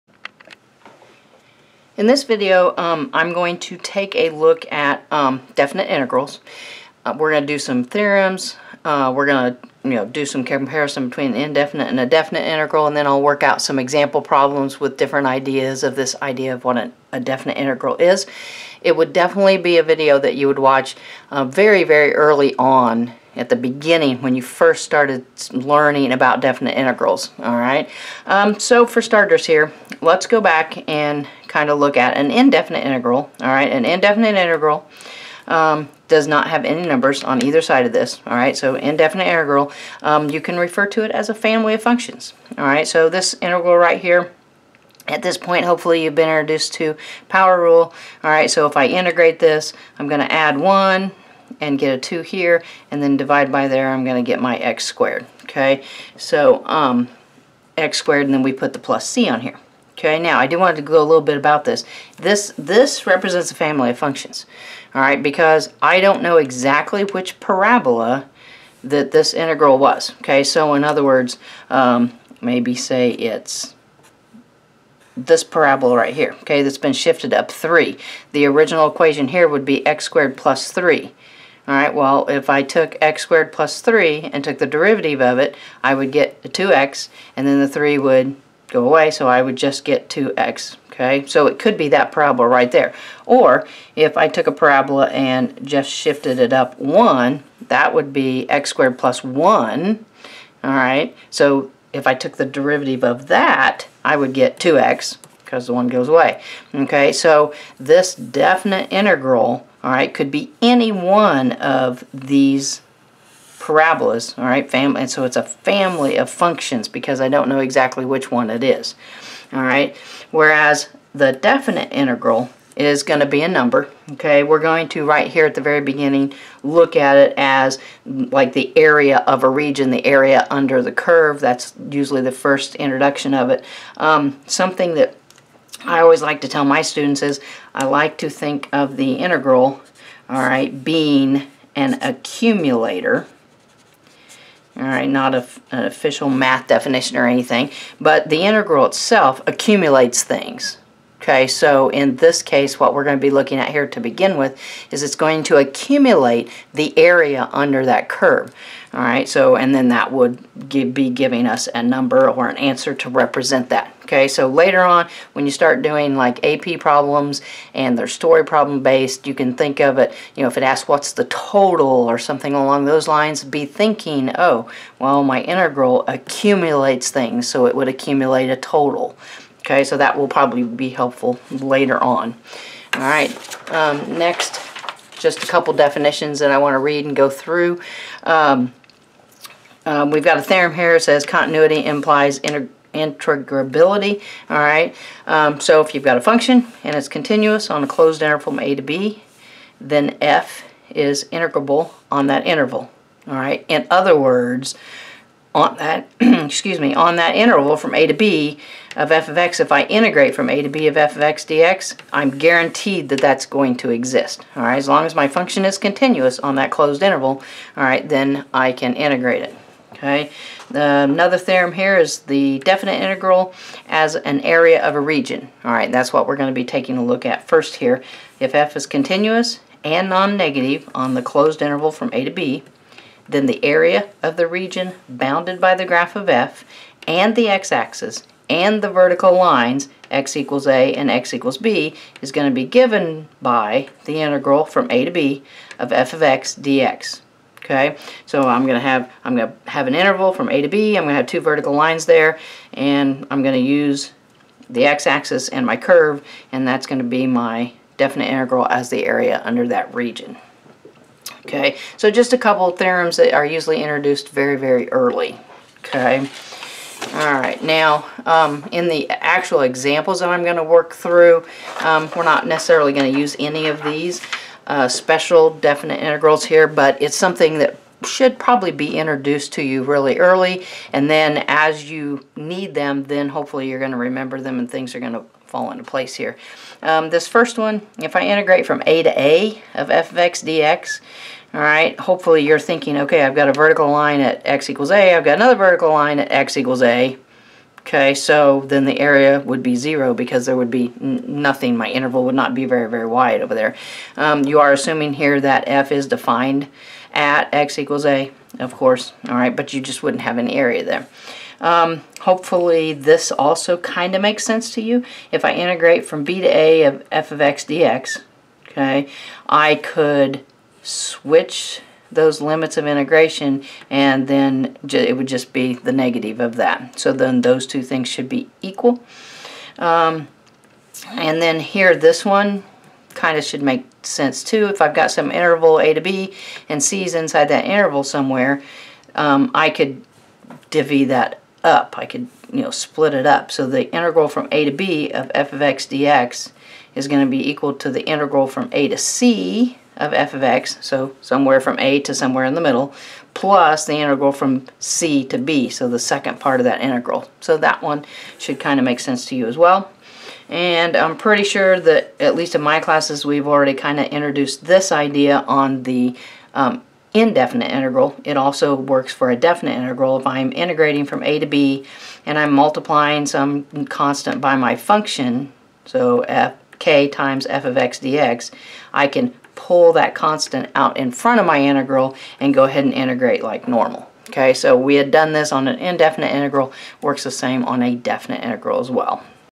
A Brief Introduction to the Definite Integral (Part 1 of 2)
This video compares the Definition of an Indefinite Integral to the Definite Integral.  It also discusses 2 theorems: "Continuity Implies Integrability" and "The Definite Integral as the Area of a Region" along with giving a discussion of some special integrals.

This video is Part 1 of 2 on this topic.